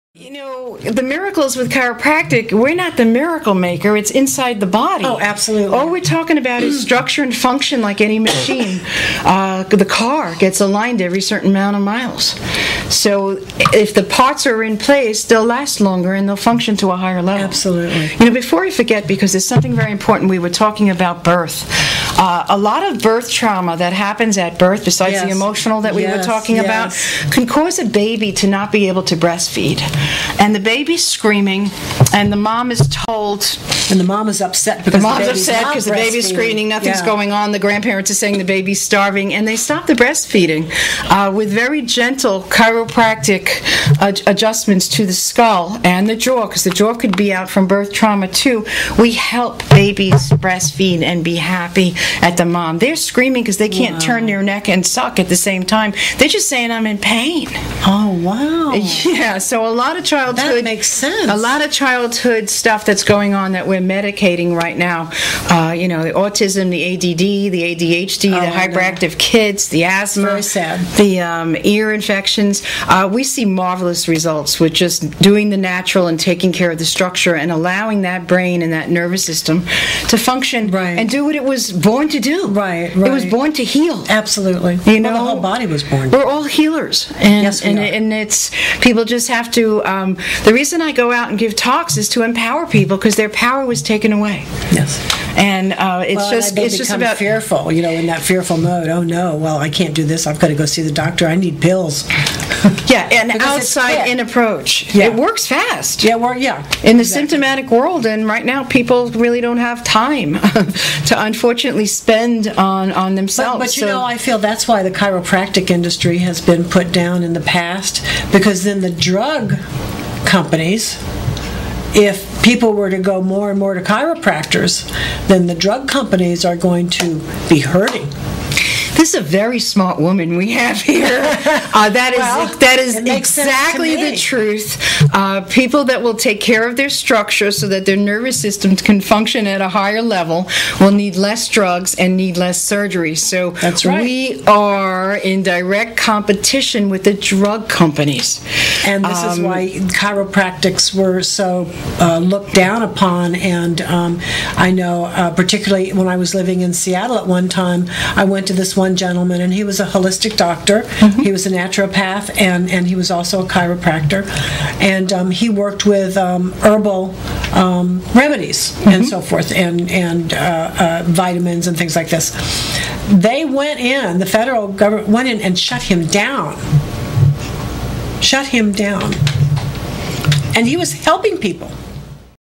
dr. lynn migdal 6_xvid.avi
Getting to the true cause or just treating symptoms, Natural Information on Birth Trauma, and Natural Healing for Children.